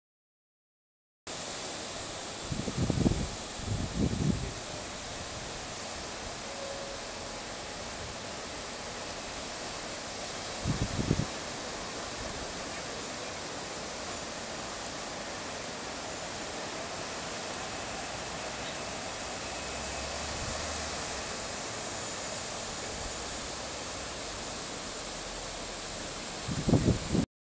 Solarium Hotel Montemar - Benidorm - SemanaSanta2013 - Vistas relajantes
¿ Quieres vivir estas sensaciones en Semana Santa?
Ven al HotelMontemar - Benidorm...
¡¡ y súbete a nuestro Solarium !! :)
(6ª planta)